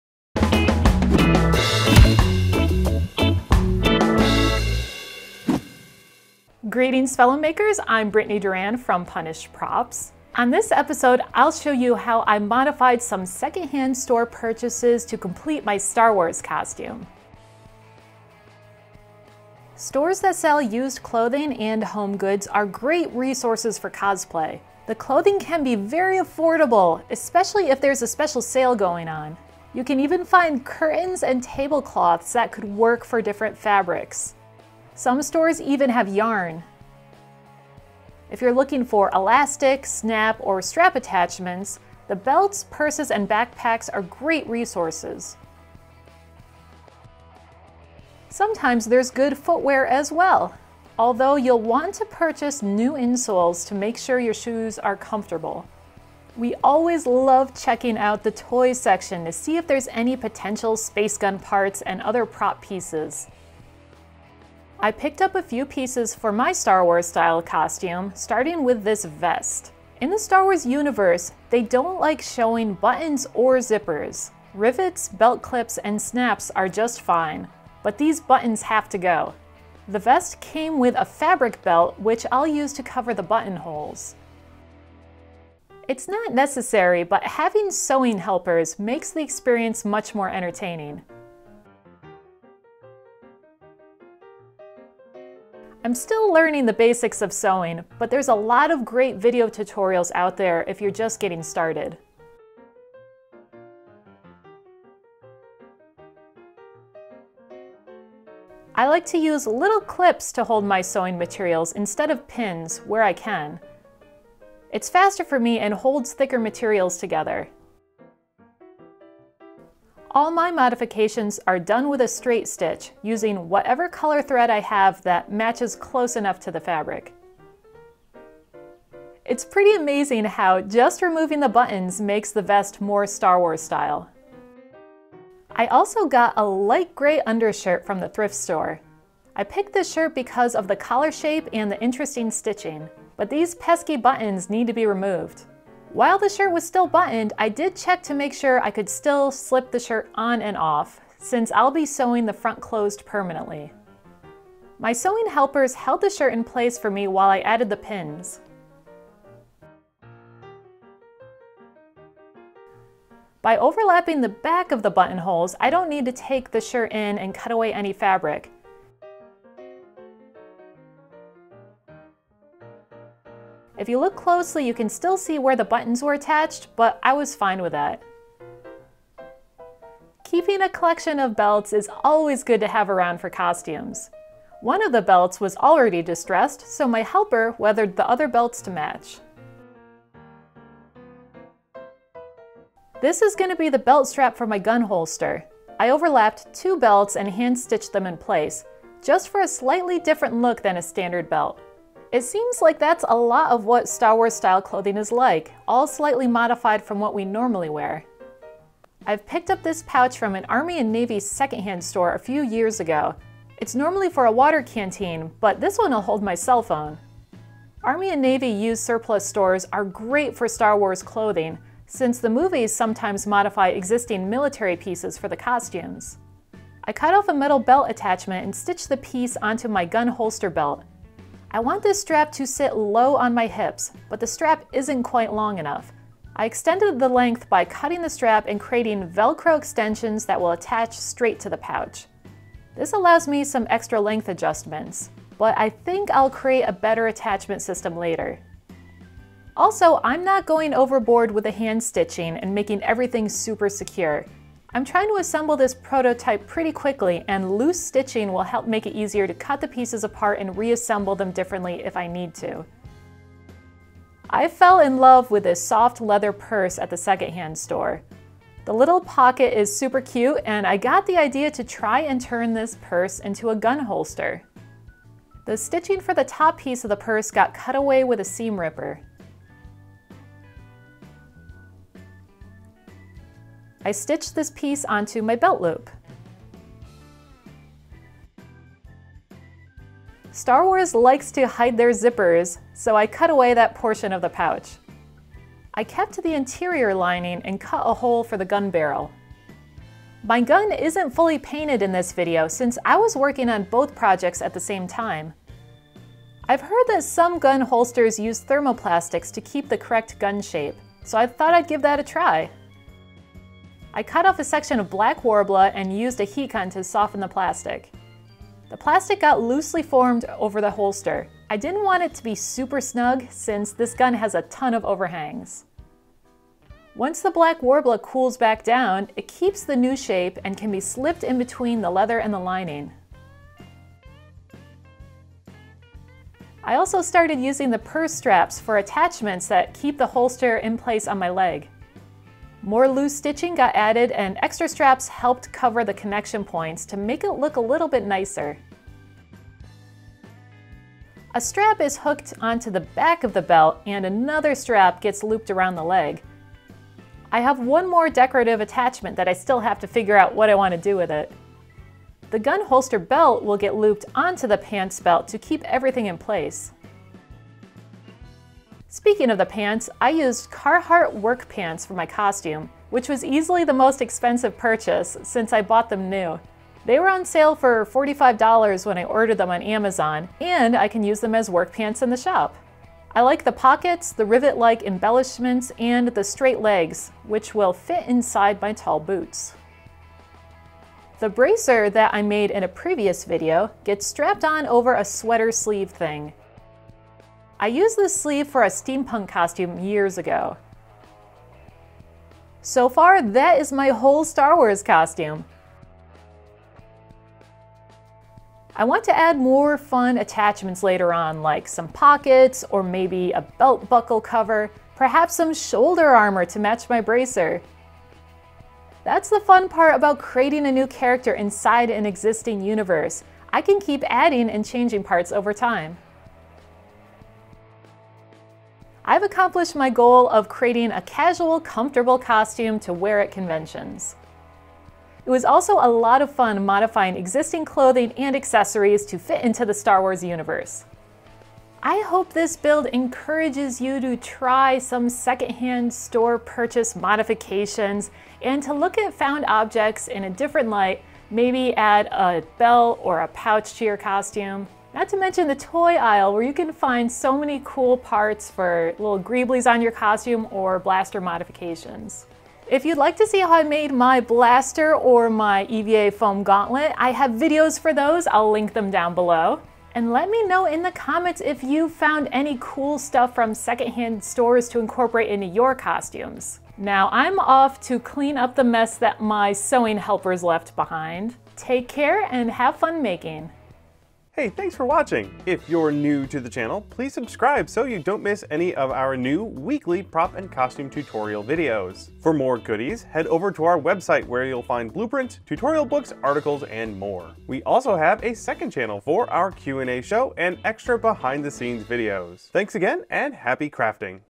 Star Wars Cosplay Clothing - Save Big With Thrift Store Mods!
Britt picks up some clothes and accessories from a thrift store and modifies parts to match the Star Wars universe. Altering second-hand store items is super useful for cosplay!

Tools and Materials Used:

Janome Sewing Machine

Seam Ripper

Craft and Sewing Clips

Carhartt Women's 1889 Slim Fit Canvas Pants